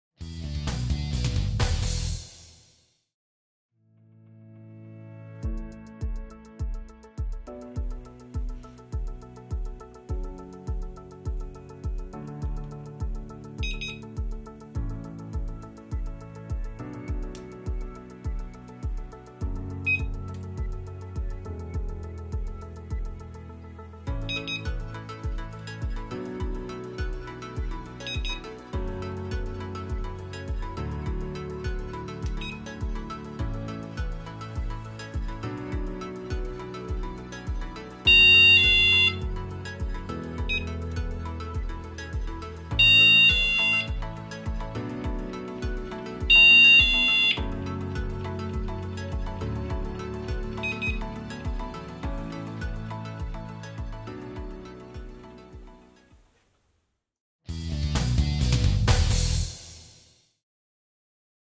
Tiiwee X4 - Connexion des capteurs de porte
Bien que le détecteur de mouvement X4 soit une alarme puissante et complète, il peut même être étendu avec de nombreux accessoires de la gamme Tiiwee Home Alarm System. Cette vidéo montre la procédure simple pour connecter sans fil les capteurs de porte et de fenêtre. Veuillez également consulter le mode d'emploi détaillé fourni avec le X4. Il décrit toutes les procédures en détail.